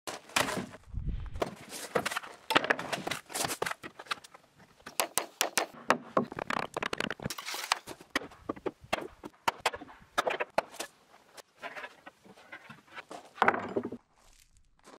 How to dismantle a europallet in 15 seconds
This is a version of the longer video on how to take apart an EPAL pallet. you can watch the full